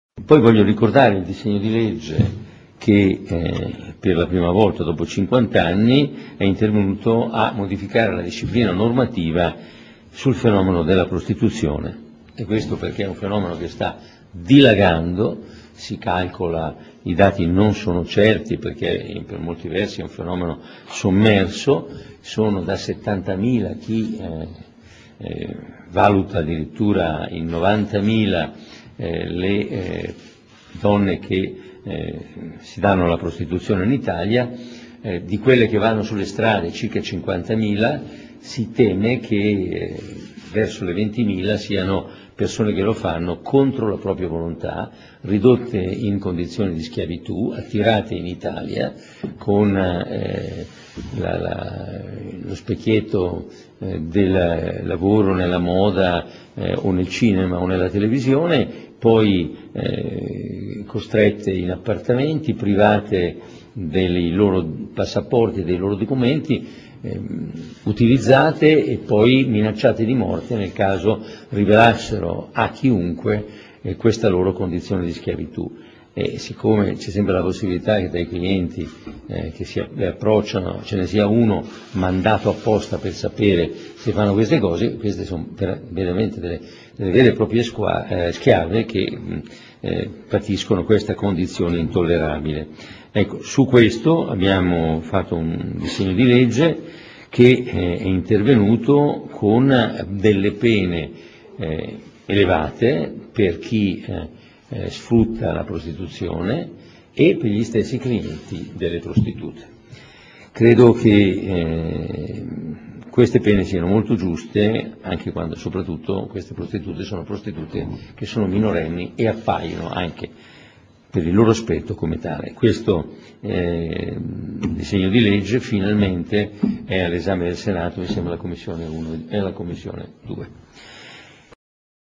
Berlusconi e il ddl prostituzione
il premier parla del provvedimento con il ministro per le Pari opportunità Mara Carfagna e, si dice d'accordo sulle pene ai clienti delle prostitute.